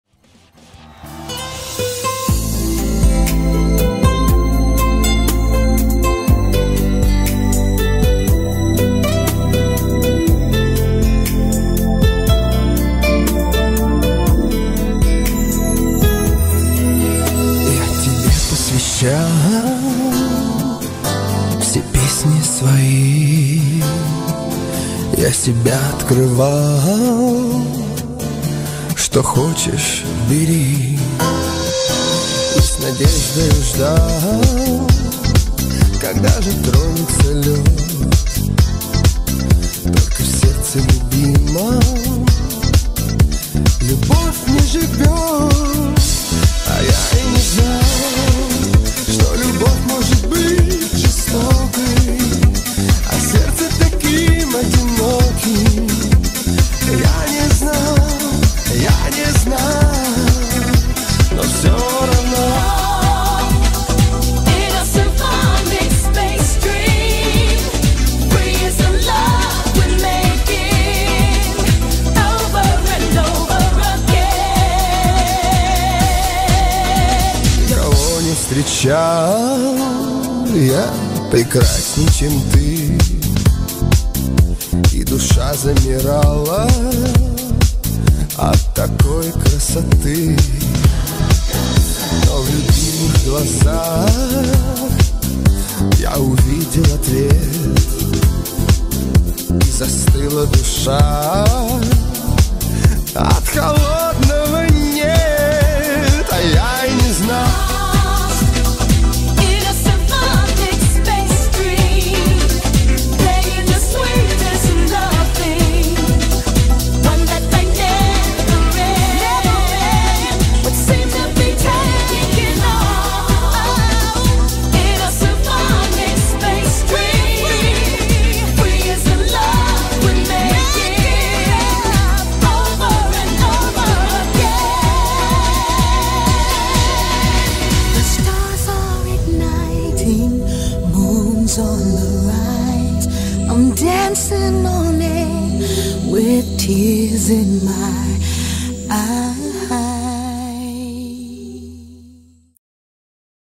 Плагиат: Space vs Филипп Киркоров
О скандале с украденной Филиппом Киркоровым у француза Дидье Маруани (группа Space) песней слышали все. Вот наглядное сравнение...